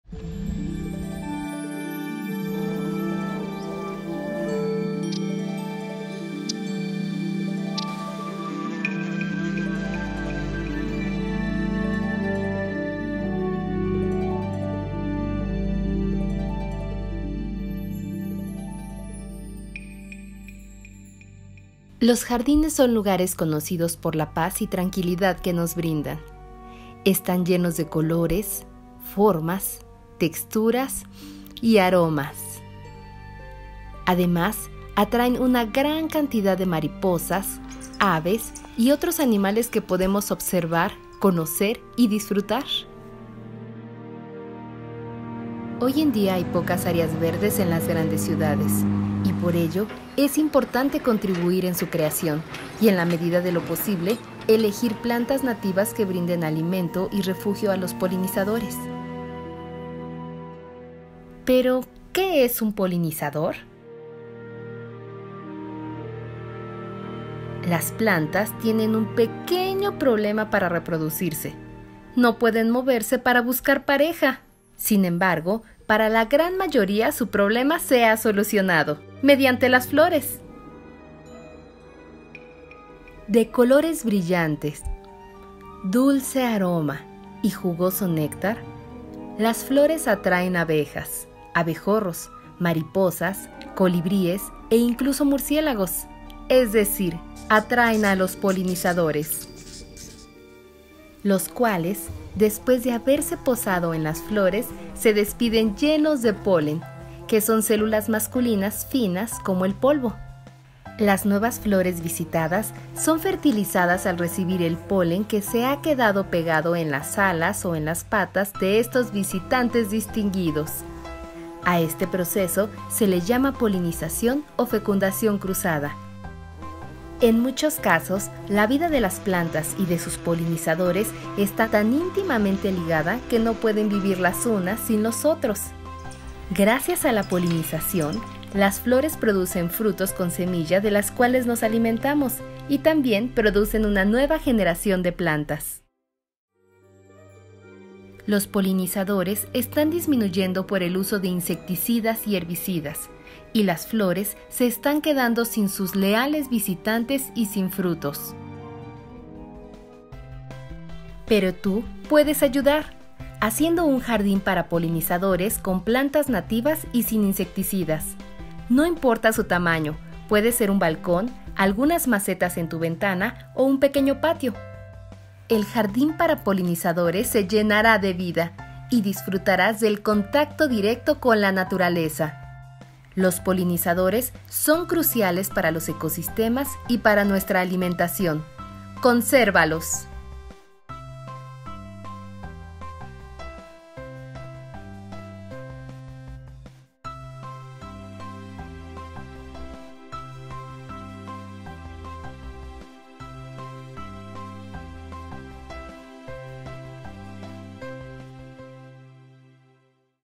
La magia de los Polinizadores
Los polinizadores son esenciales en nuestro ambiente. Abejas, abejorros, mariposas, colibríes, murciélagos nectarívoros, entre otros, al alimentarse de néctar mueven el polen de una flor a otra asistiendo a su reproducción y por ende a la formación de frutos. Los polinizadores son responsables de la reproducción sexual de más del 80% de las plantas vasculares terrestres.